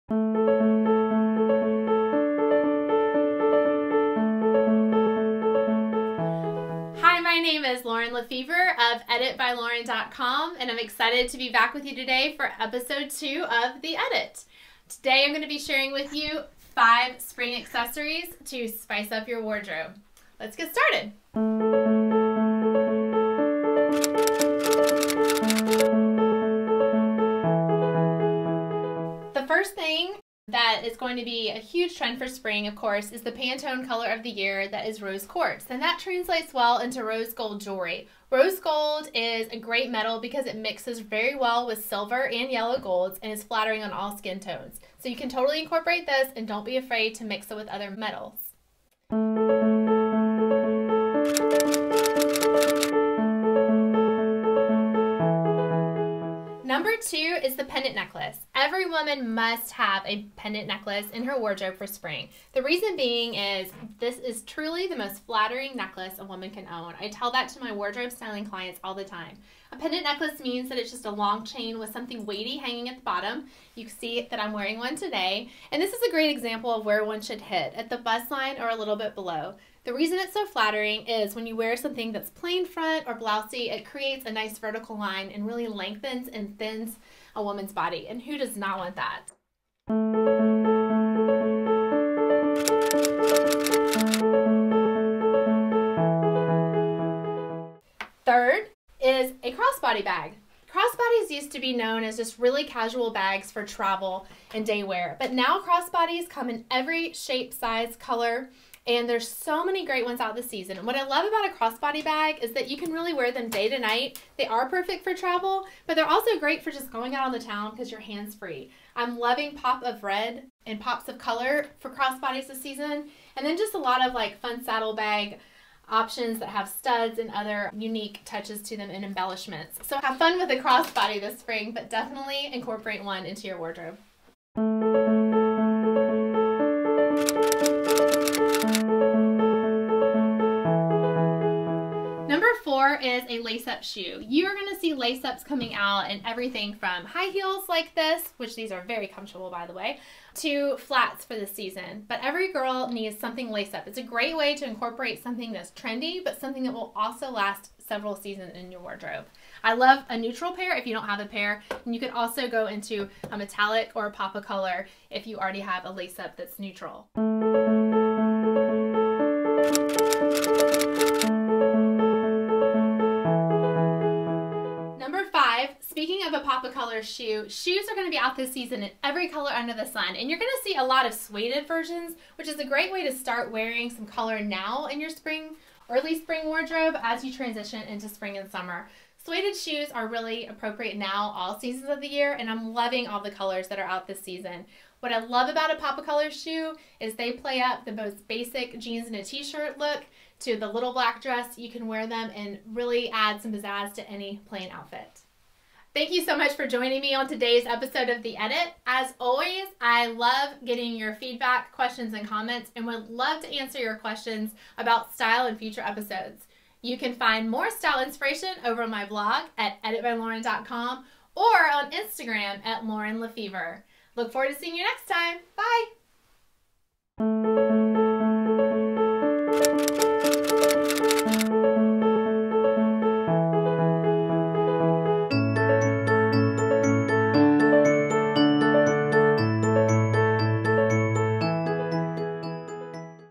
'The Edit' Ep. 002 | 5 Accessory Trends For Spring
Welcome to Episode 002 of "The Edit".
5 Accessory Trends for Spring

SNAPCHAT | Like many of you, I LOVE Snapchat.
You can add me by Username :: LaurenLefevre

TIME STAMPS & SHOPPABLE LINKS
0:23 - Rose Gold Jewelry :: short necklace long necklace
0:44 - The Pendant Necklace
1:31 - Cross-Body Bag :: red bag similar brown bag
2:15 - Lace Up Shoe :: and
2:48 - Pop of Color Shoe :: and

Filmed & Edited by JaWaVi Films | Athens, Ga